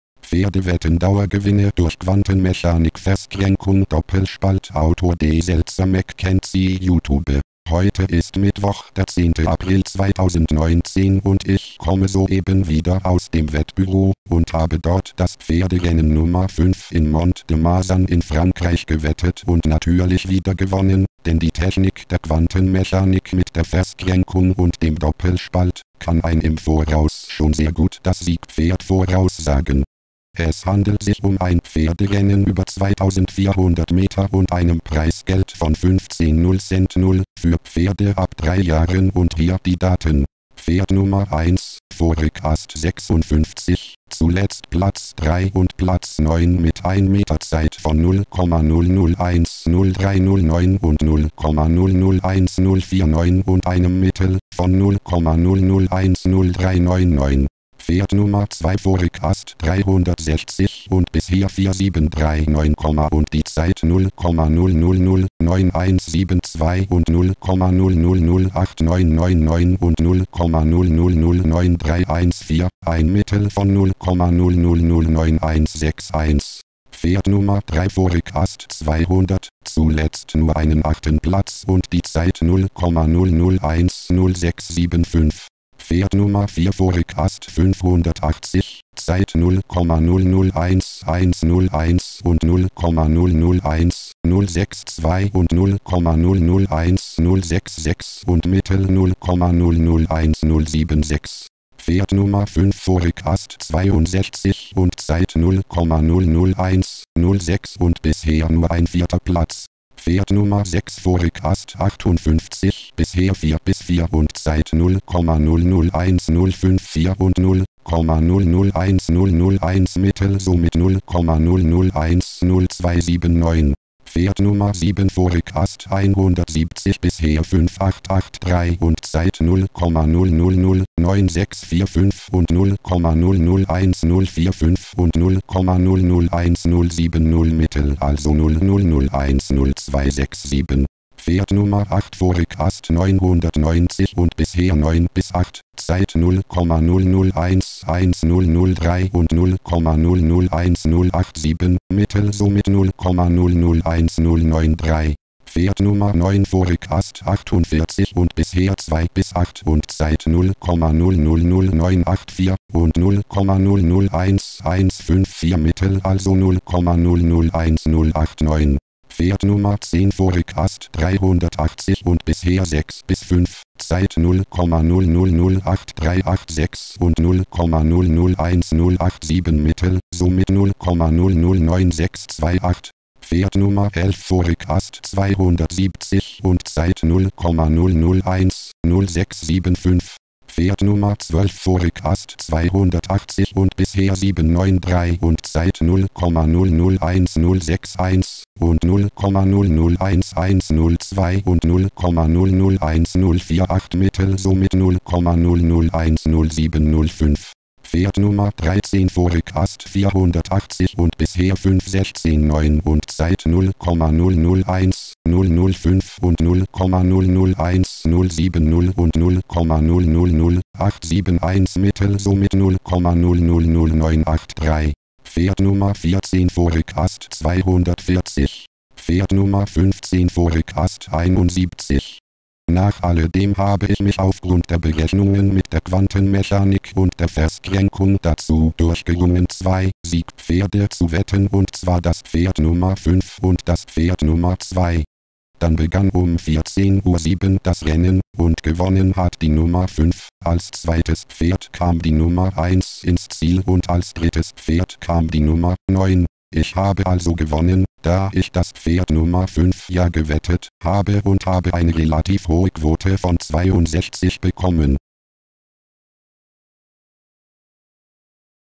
Pferdewetten Dauergewinne durch Quantenmechanik Verschränkung Doppelspalt
Author Dr. D. Selzer-McKenzie

Heute ist Mittwoch der 10. April 2019, und ich komme soeben wieder aus dem Wettbüro, und habe dort das Pferderennen Nummer 5 in Mont de Marsan in Frankreich gewettet, und natürlich wiedergewonnen, denn die Technik der Quantenmechanik mit der Verschränkung und dem Doppelspalt kann ein im Voraus schon sehr gut das Sieg Pferd voraussagen.

Es handelt sich um ein Pferderennen über 2400 m und einem Preisgeld von 15.000 € für Pferde ab drei Jahren, und hier die Daten:

Pferd Nummer 1, Forecast 56, zuletzt Platz drei und Platz neun, mit 1 m Zeit von 0,0010309 und 0,001049, und einem Mittel von 0,0010399

Pferd Nummer 2 Forecast 360, und bis hier 4-7-3-9, und die Zeit 0,0009172 und 0,0008999 und 0,0009314, ein Mittel von 0,0009161

Pferd Nummer 3 Forecast 200, zuletzt nur einen achten Platz und die Zeit 0,0010675

Pferd Nummer 4 Forecast 580, Zeit 0,001101 und 0,001062 und 0,001066, und Mittel 0,001076

Pferd Nummer 5 Forecast 62 und Zeit 0,00106 und bisher nur ein vierter Platz

Pferd Nummer 6 Forecast 58, bisher 4-4, und Zeit 0,001054 und 0,001001 Mittel somit 0,0010279

Pferd Nummer 7 Forecast 170 bisher 5-8-8-3 und Zeit 0,0009645 und 0,001045 und 0,001070 Mittel also 0,0010267

Pferd Nummer 8 Forecast 990 und bisher 9-8, Zeit 0,0011003 und 0,001087, Mittel somit 0,001093

Pferd Nummer 9 Forecast 48 und bisher 2-8, und Zeit 0,000984 und 0,001154 Mittel also 0,001089

Pferd Nummer zehn Forecast 380 und bisher 6-5, Zeit 0,0008386 und 0,001087 Mittel somit 0,009628

Pferd Nummer elf Forecast 270 und Zeit 0,0010675

Pferd Nummer zwölf Forecast 280 und bisher 7-9-3 und Zeit 0,001061 und 0,001102 und 0,001048 Mittel somit 0,0010705

Pferd Nummer 13 Forecast 480 und bisher 5-16-9 und Zeit 0,001005 und 0,001070 und 0,000871 Mittel somit 0,000983

Pferd Nummer 14 Forecast 240

Pferd Nummer 15 Forecast 71.

Nach alledem habe ich mich aufgrund der Berechnungen mit der Quantenmechanik und der Verschränkung dazu durchgerungen zwei Sieg Pferde zu wetten und zwar das Pferd Nummer 5 und das Pferd Nummer 2.

Dann begann um 14:07 Uhr das Rennen, und gewonnen hat die Nummer 5, als zweites Pferd kam die Nummer 1 ins Ziel und als drittes Pferd kam die Nummer 9.ich habe also gewonnen, da ich das Pferd Nummer 5 ja gewettet habe und habe eine relativ hohe Quote von 62 bekommen.